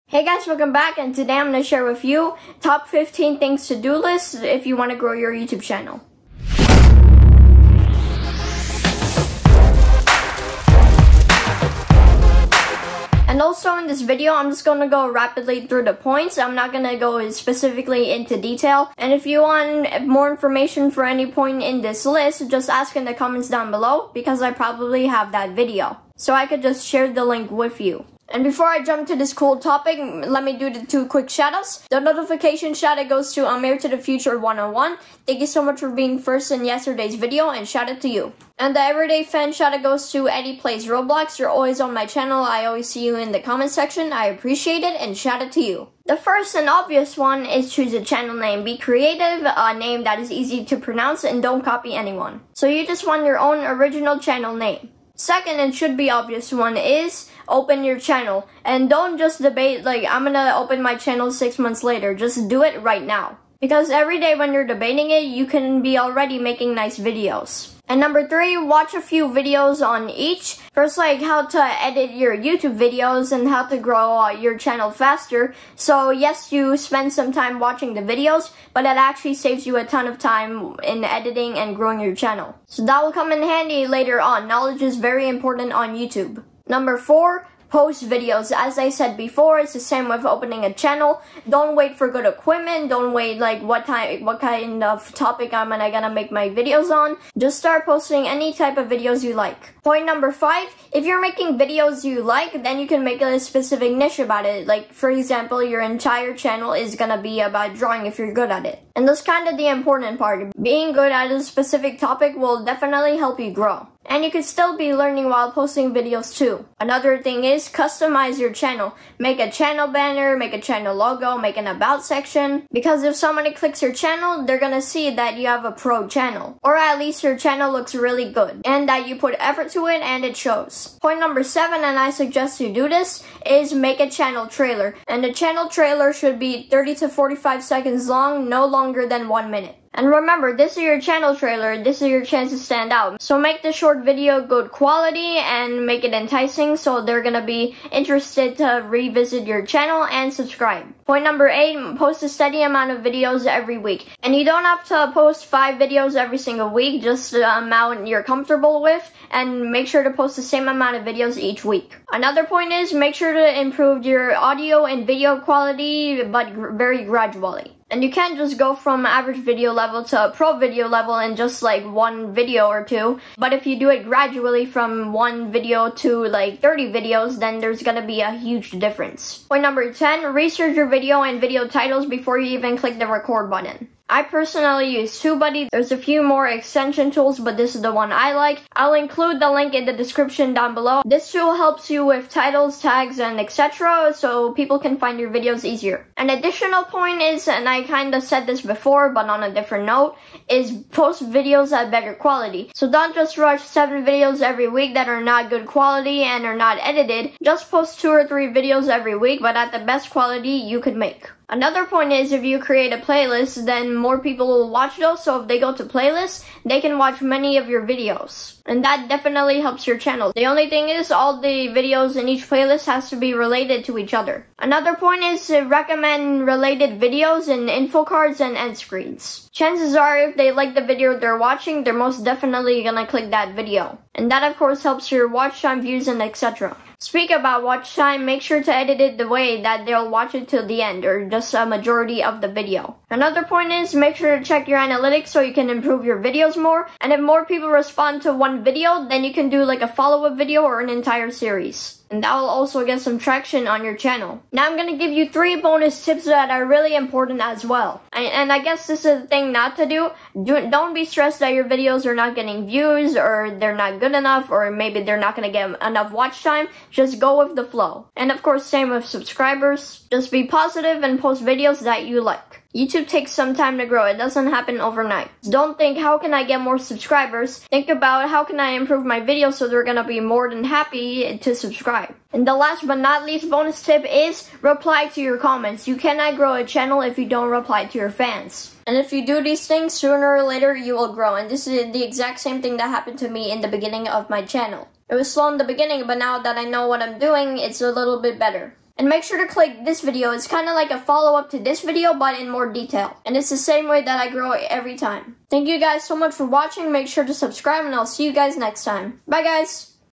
Top 15 Things to do list for Anyone Who Wants to Grow a Youtube Channel
Top 15 to do list for Anyone Who Wants to Grow a Youtube Channel | ChrisGTXvideos
This is Just a Quick List with 15 Things to Do List if You Really Wanna Grow on Youtube , get Views , Subscribers and get Noticed on Youtube.

I'll go thru this list Rapidly so If You want me to Explain and Point in more Detail Just Ask in Comments and I'll Share that Specific Video from the To Do list with You.
Growing on Youtube is Actually not That Difficult but Definitely there are Steps We Need to Take Since There is A lot of Youtube Creators so We Have Chance to stand Out.

I Created this Youtube Things to Do list from My own Experience and I Hope it Will Help You bto Grow Your Channel.
First Actually Open a Youtube Channel , Waiting is a Waste of Time.
Posting a lot of Videos a week is Great but whats Even More important is to Post Videos Regularly.
Another Important thing to do is Reply to ALL or Most of Your fans Comments on Your Channel , It's Not Only a Nice and Correct thing to do but You Can't really Grow a Youtube Channel if You Don't reply to Your Fans.
There is a lot of Things to do on Youtube to grow , I Find These 15 Top Crucial
Enjoy The rest of The Tips in this Video.